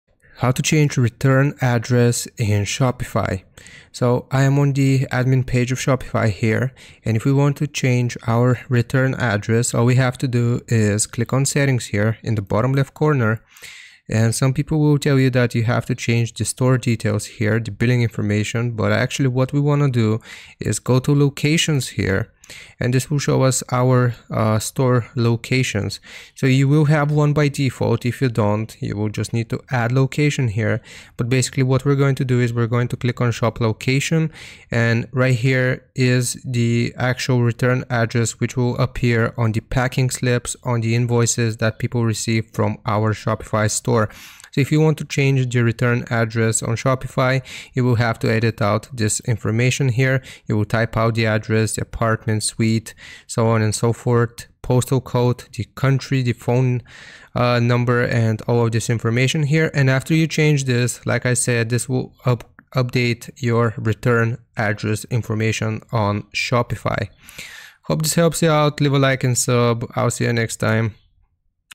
How To Change Return Address In Shopify
In this video I show you how to change return address in Shopify. If you want to learn how to change the address people return Shopify orders to then this is the tutorial for you.